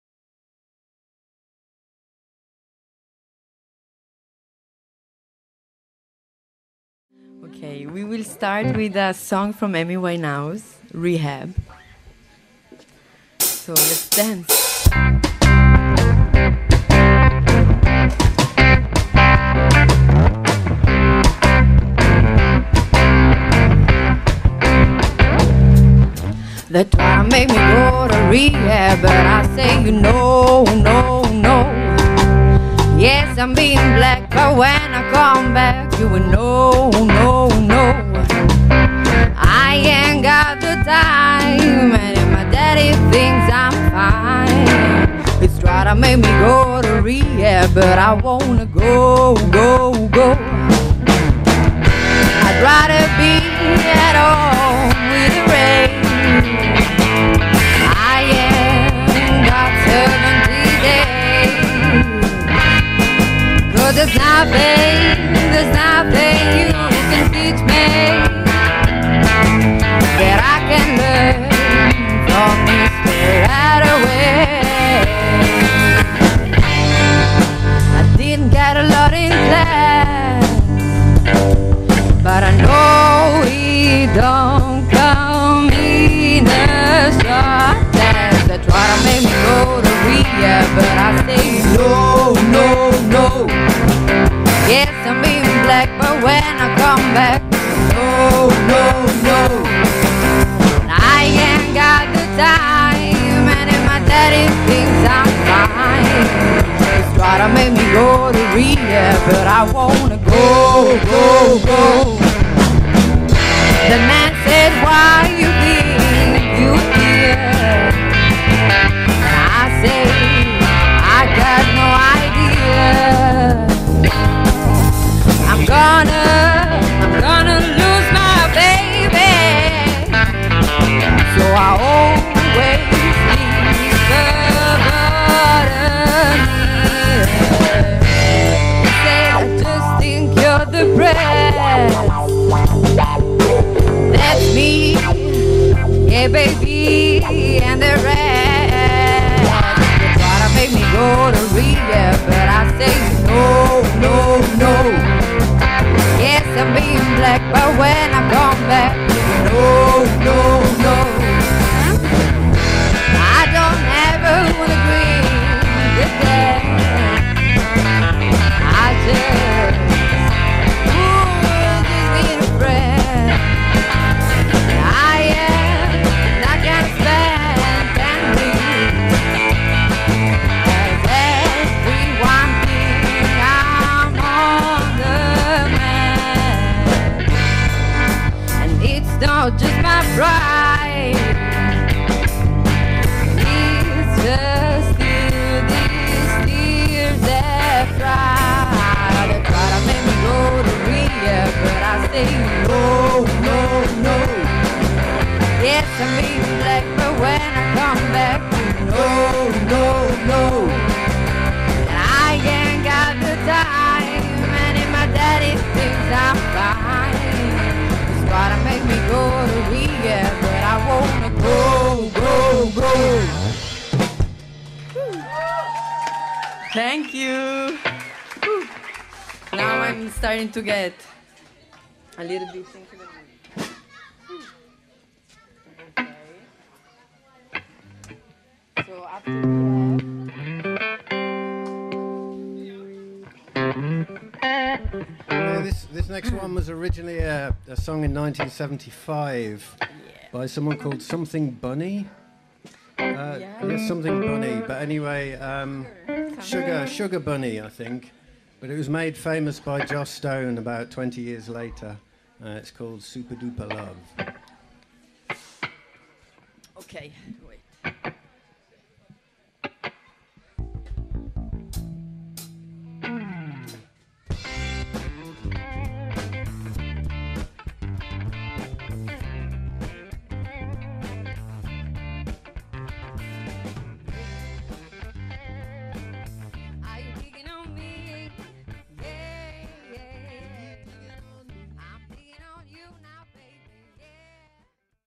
Soul Cypher Live Performance @ Wellcafe
Soul cypher is a band from Auroville comprising of Edo on lead guitar, Matthew on bass guitar/vocals, Margharita on vocals and Mehul on drums. They performed last saturday at wellcafe and played a good collection of rock music and other styles entertaining the crowd. Here is the audio recording of their live performance